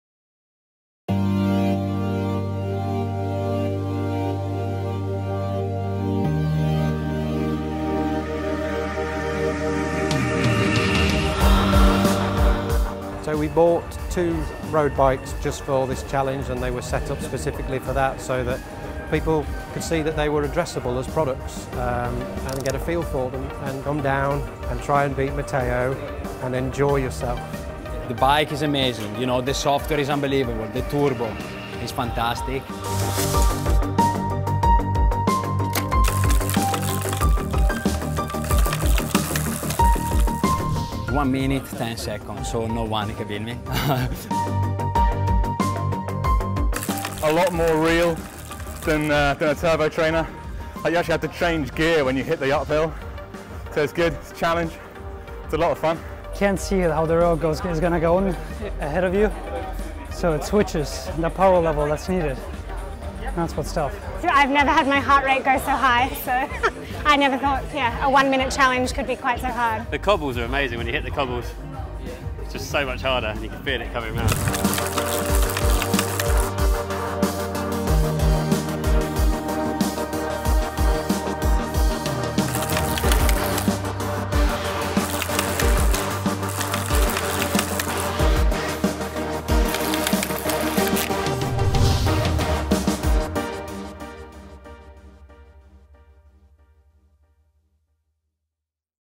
The Dassi City Challenge 2015 - Qualifying Round
Former Tour De France cyclist Matteo Carrara threw down the gauntlet for the Dassi bikes inter-company challenge.

Linklaters Event - Qualifying Round